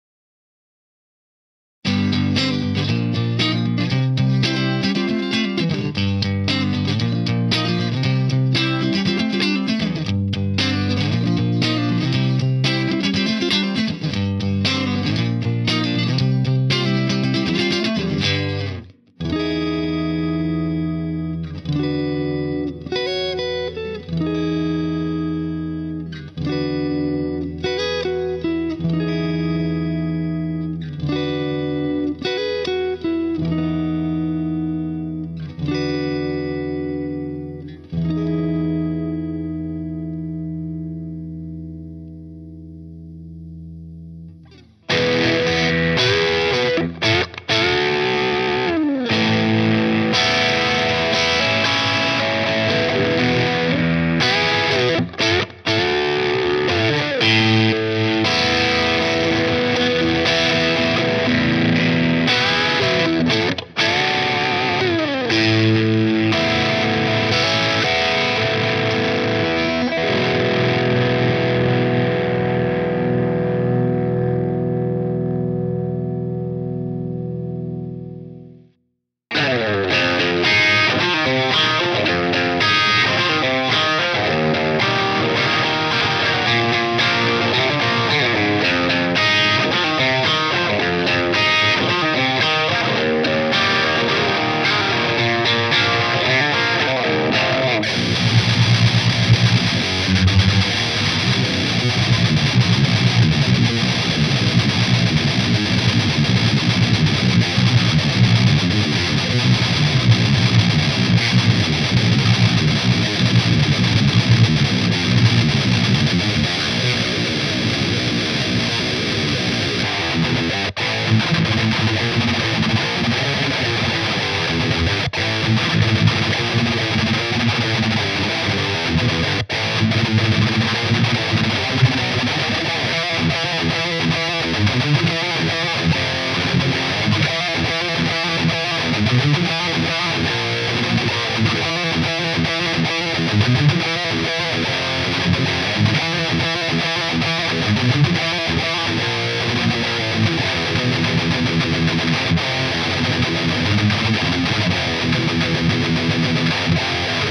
EHV 50S - DMN
EHV 50S based on EVH 2x12 Stealth, loaded with Celestion G12M Heritage/G12-EVH, G12H Heritage 75Hz, Jensen Electric Lightning 70 & HESU Demon + Secret Sauce Cab.